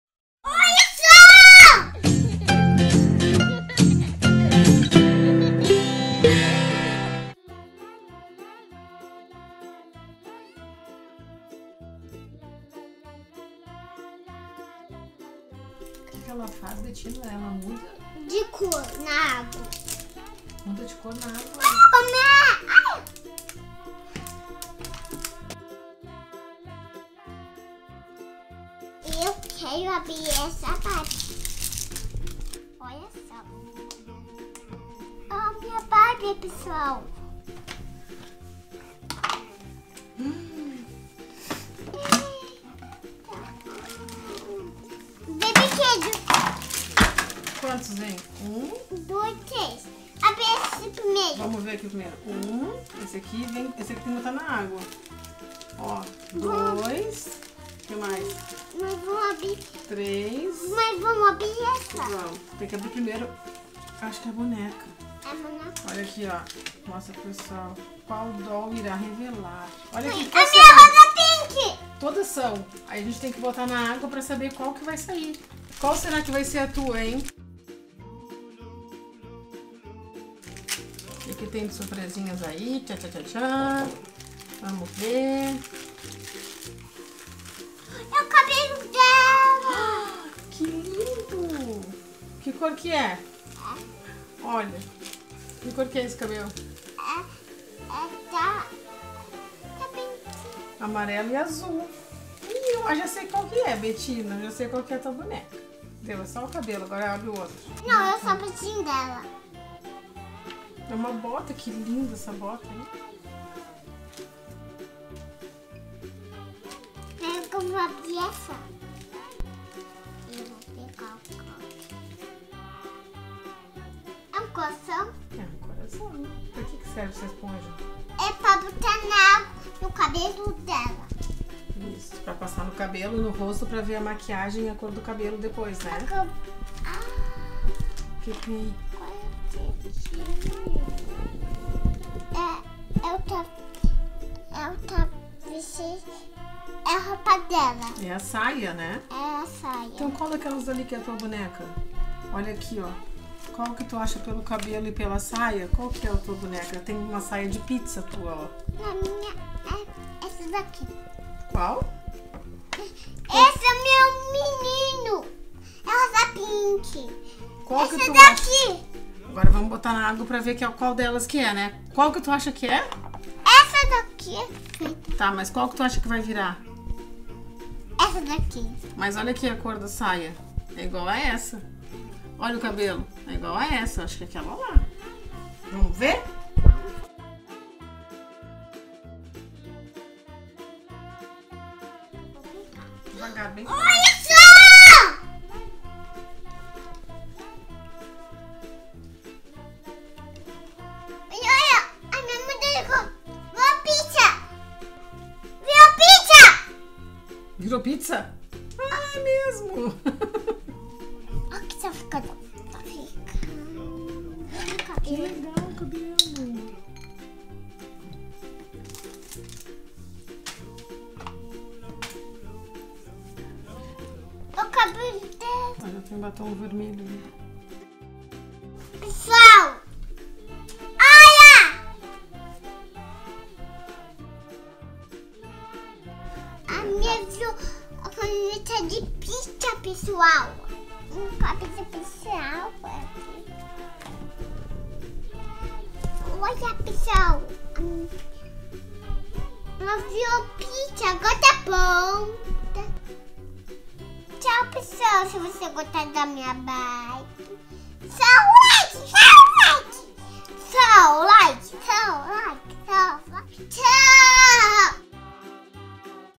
ABRINDO BARBIE COLOR REVEAL COM 7 SURPRESAS | Nova Barbie lançamento 2020 | NEW FOODIE SERIES
A Bettina abriu a nova Barbie Color Reveal, lançamento 2020 da New Foodie Series, divirta-se com essa descoberta.